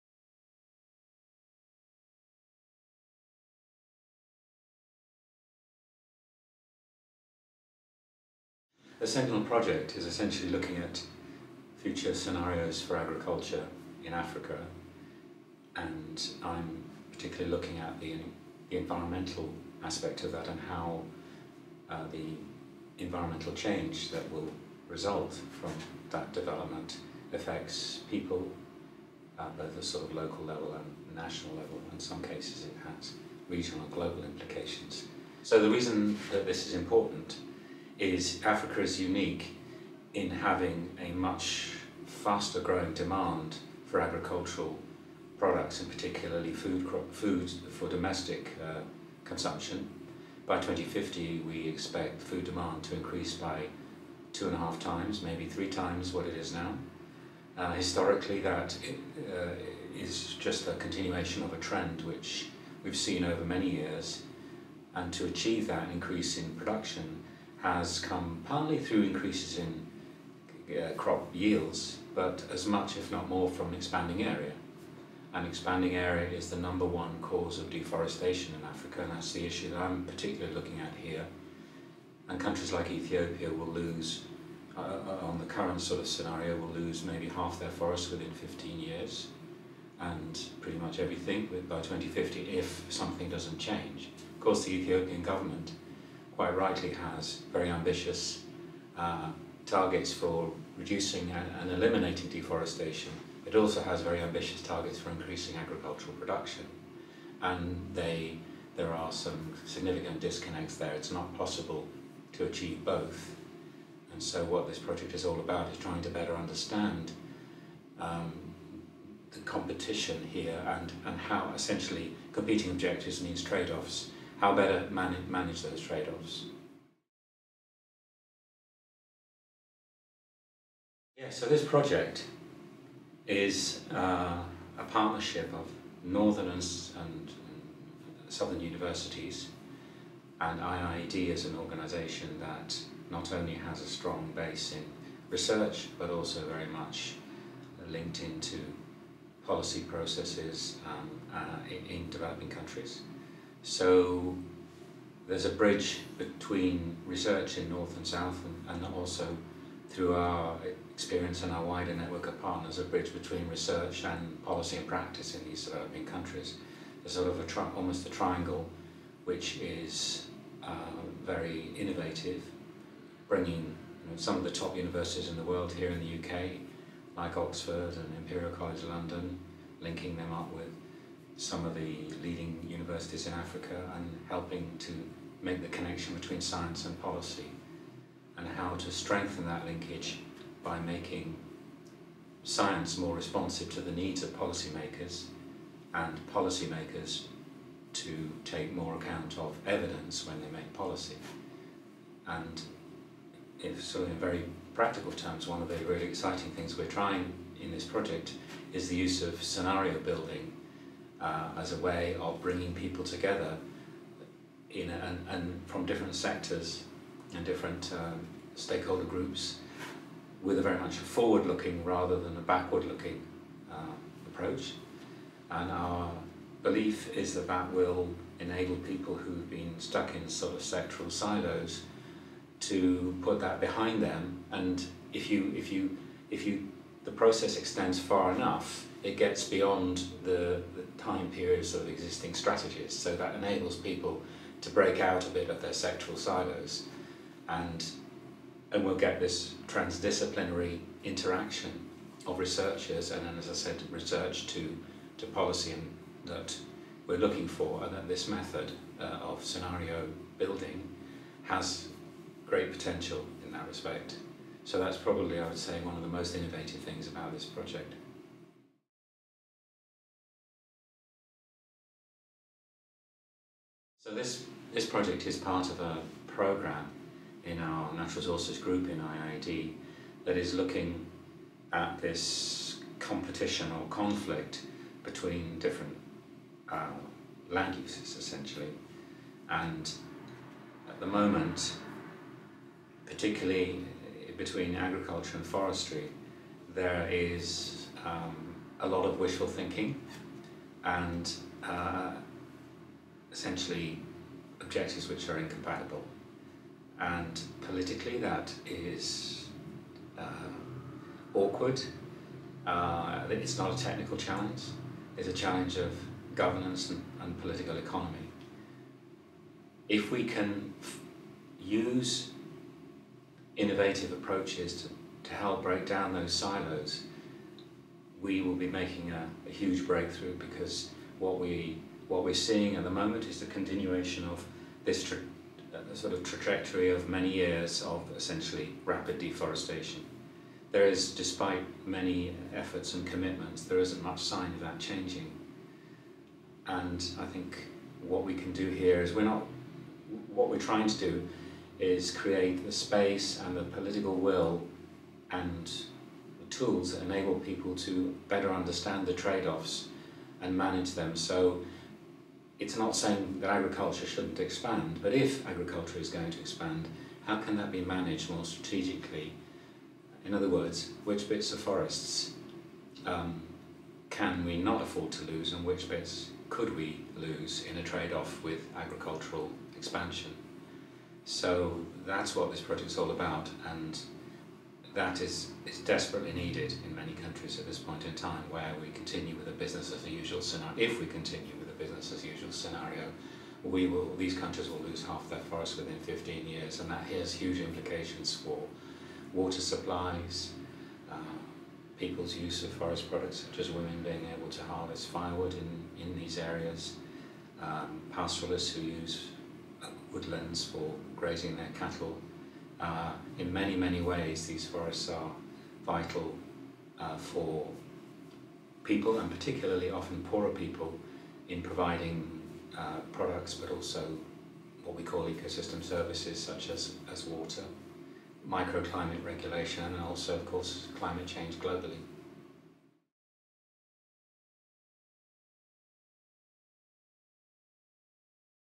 SENTINEL: Interview with Phil Franks
In December 2017, the International Institute for Environment and Development (IIED) hosted the first meeting of a new partnership aiming to tackle the challenge of producing enough food for Africa’s growing and increasingly urbanised population, while conserving vital ecosystem functions and reducing inequalities.

In this short interview, Phil Franks, senior researcher at IIED, discusses the new SENTINEL (Social and Environmental Trade-offs in African Agriculture) project. He discusses the main challenges, how the project is exciting, and its potential impact.

He says: "Africa is unique in having a much faster growing demand for agricultural products, in particularly food for domestic consumption. By 2050 we expect food demand to increase by two and a half times, maybe three times of what it is now.

"Historically, that is just the continuation of a trend that we've seen over many years. And to achieve that increase in production has come partly through increases in crop yields, but as much if not more from expanding area. And expanding area is the number one cause of deforestation in Africa."

SENTINEL is a research initiative led by IIED, supported by the Global Challenges Research Fund of the UK Research Councils.